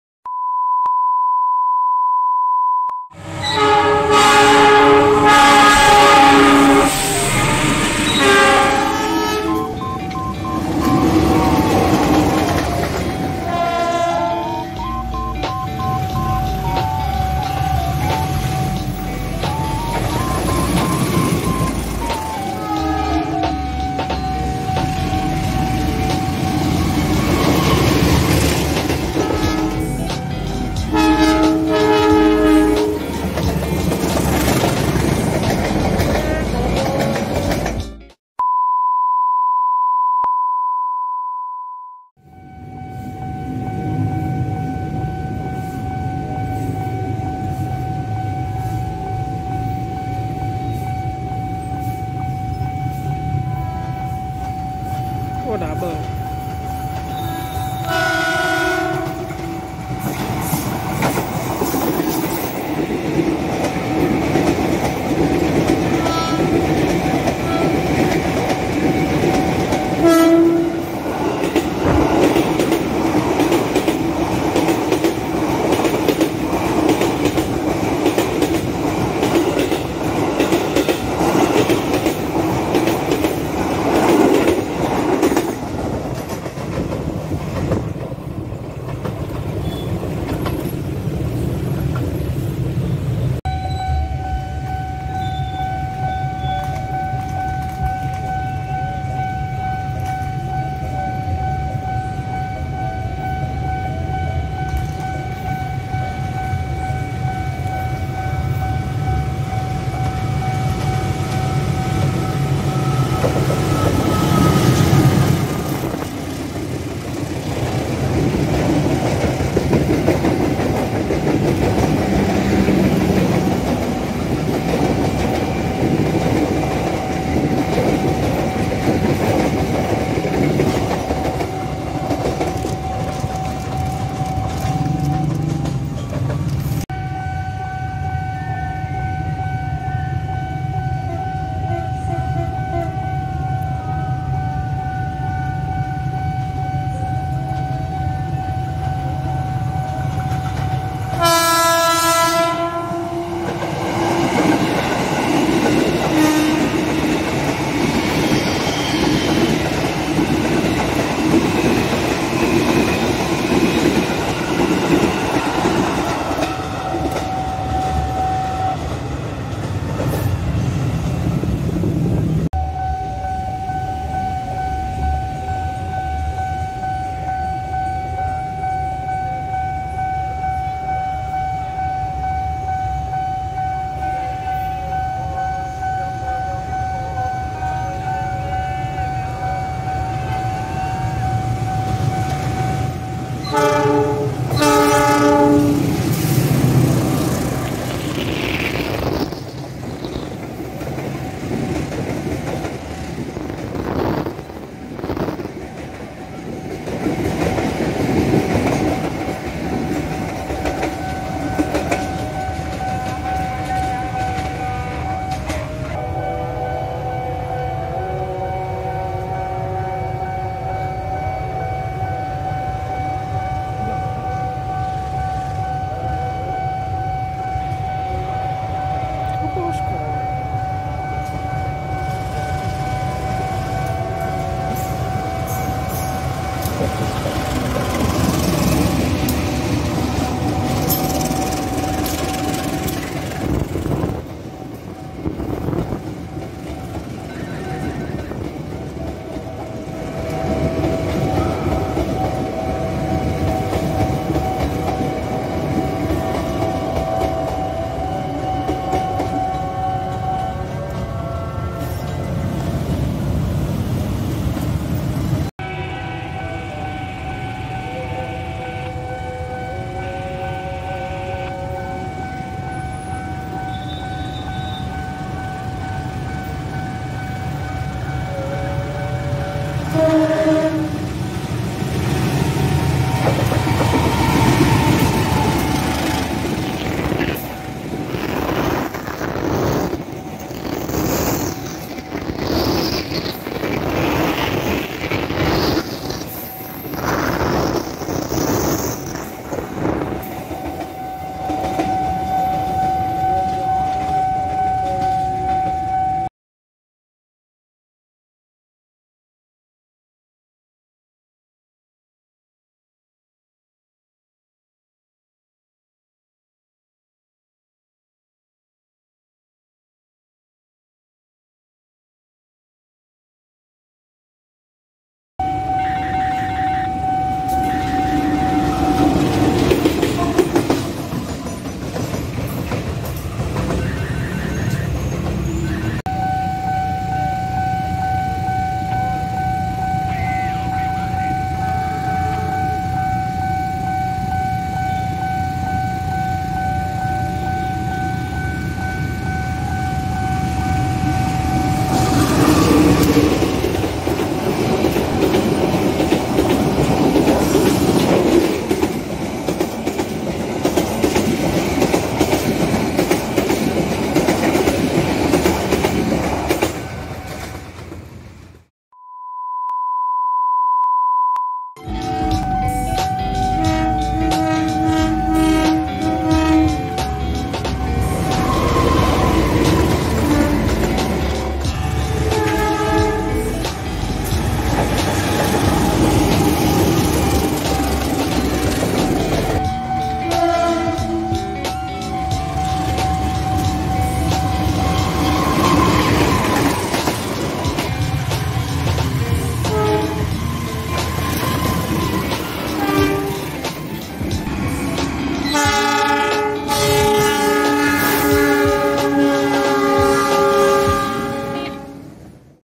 Menunggu Magrib Kereta Posko Membawa 3 Kereta Mahalnya
Moment saat menunggu adzan magrib ketemu kereta posko dengan membawa 3 kereta wisatanya